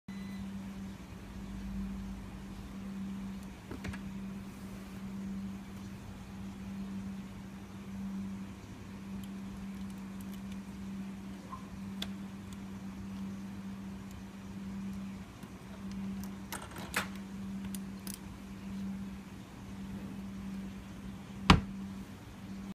How to open drawer without key
Get a u pin, shape it like a key and try, reshape it then try again..